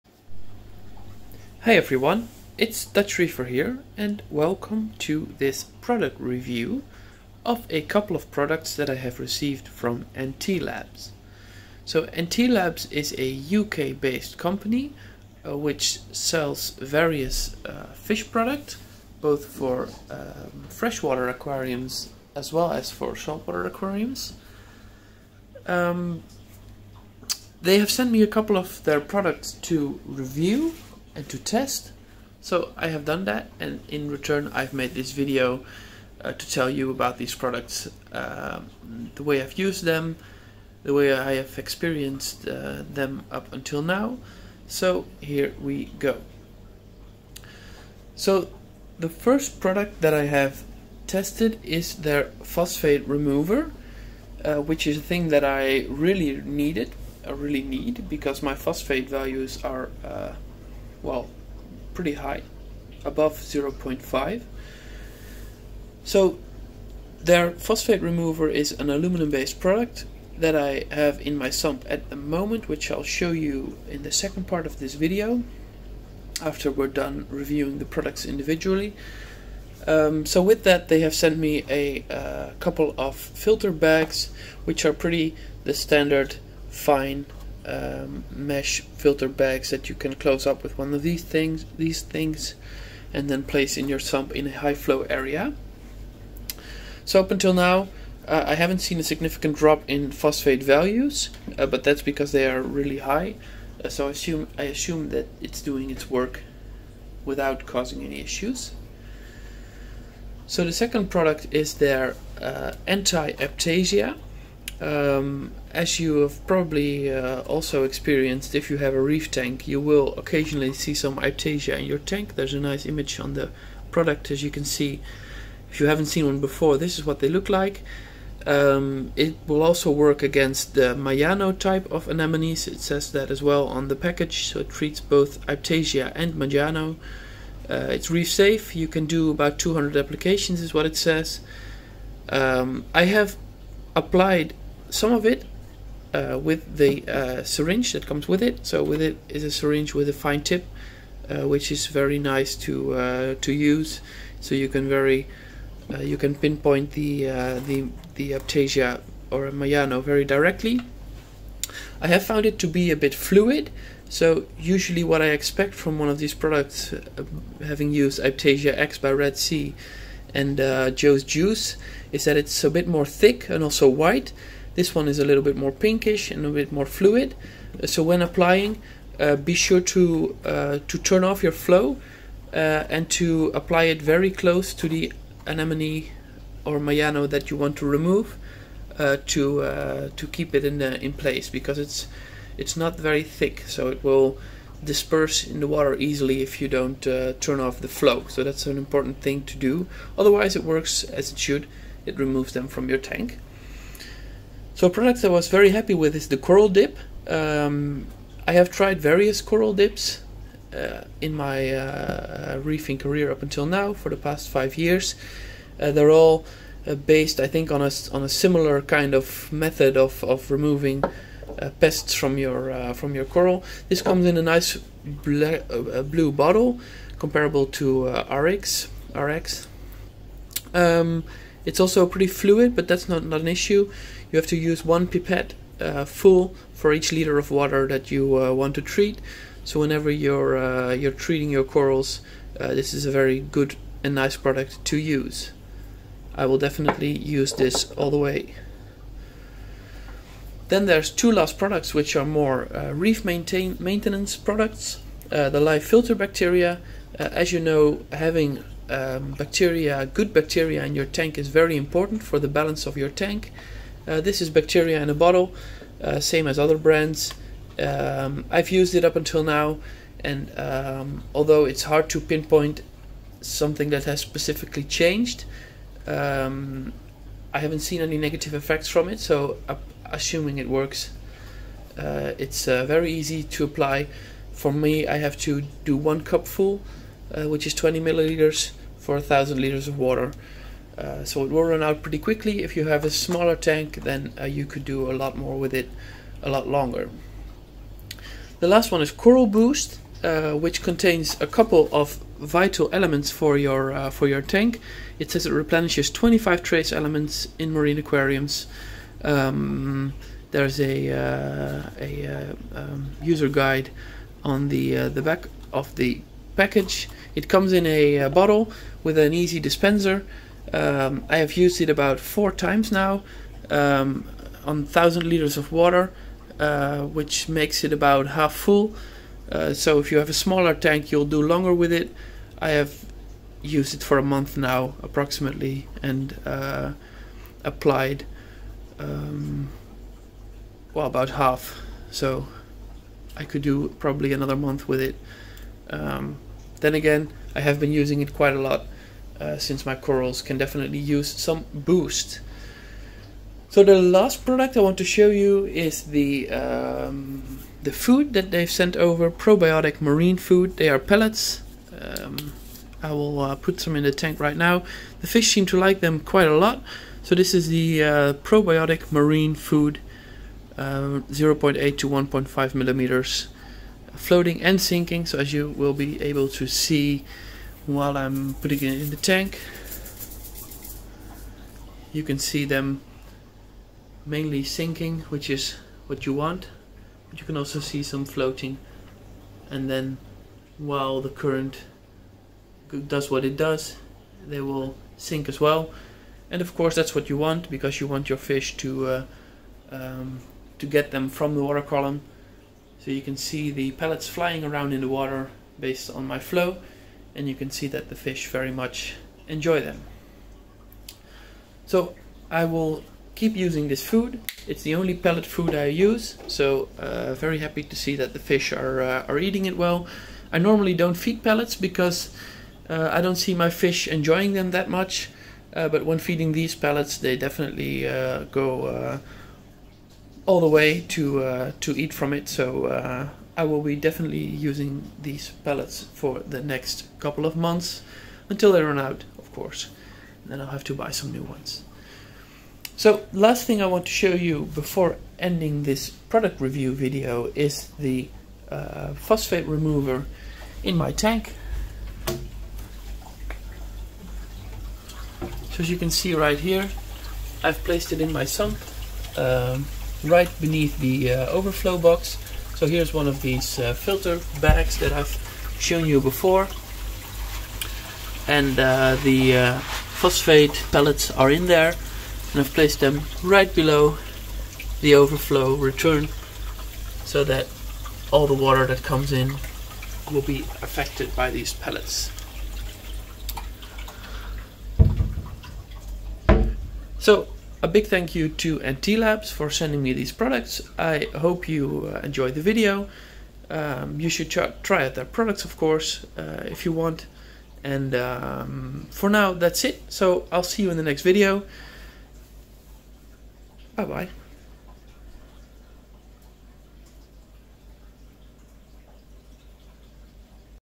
Product Review | NT Labs | Water Treatment & Food
Welcome to this product review of a number of Water Treatment products and food by NT Labs, featuring my custom built peninsula reef tank.

NT Labs Products Tested:
- Phosphate Remover
- Filter Media Bags
- Anti-Aiptasia
- Coral Dip
- Live Filter Bacteria
- Coral Boost